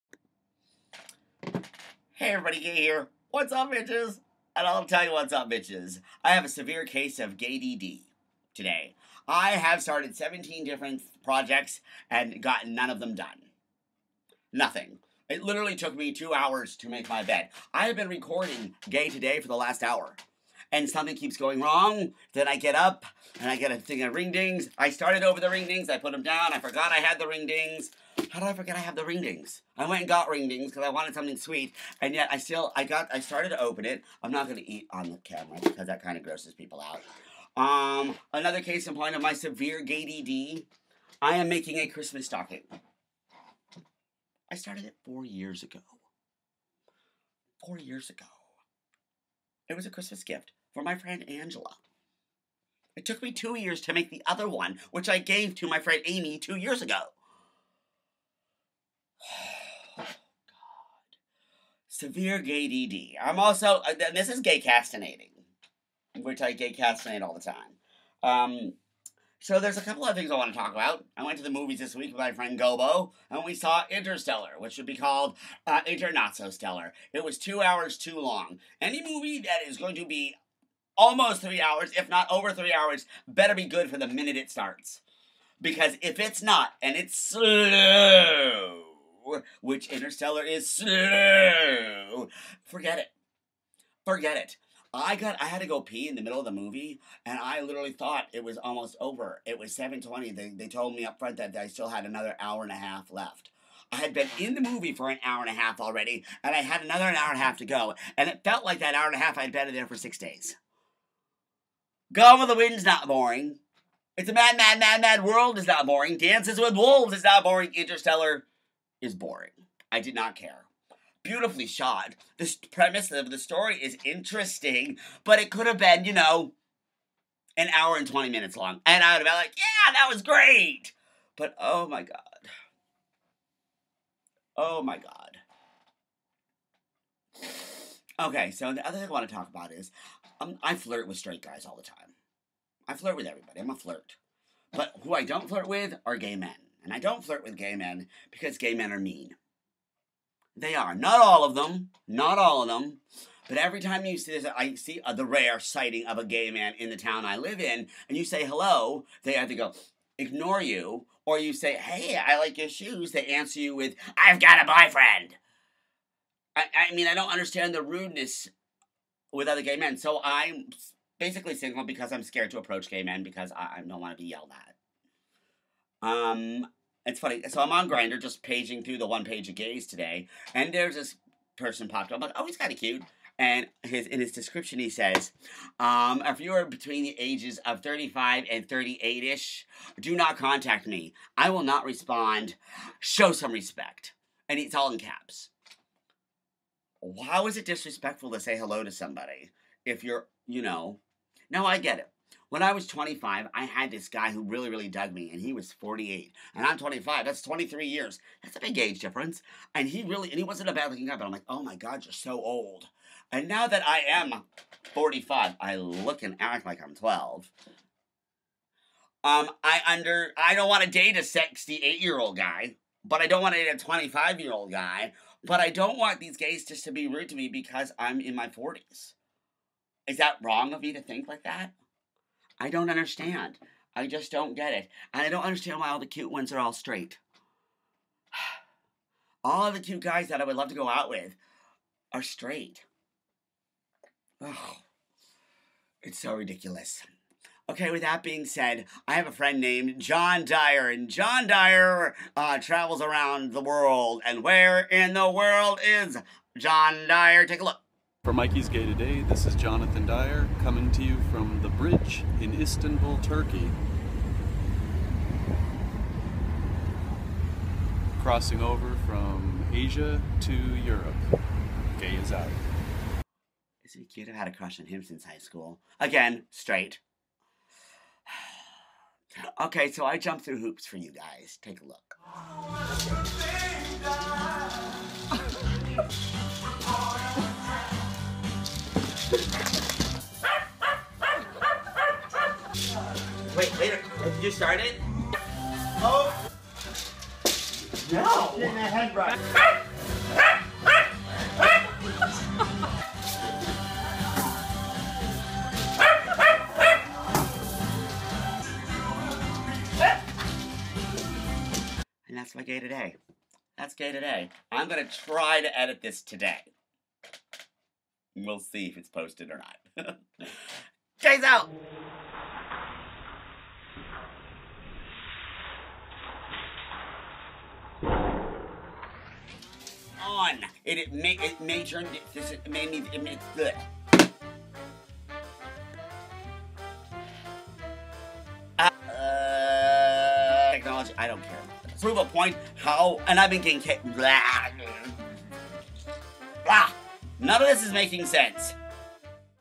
Interstellar Review and Gay Stuff
Your Gay gives his thoughts on the movie INTERSTELLAR and talks about how gay men can be mean.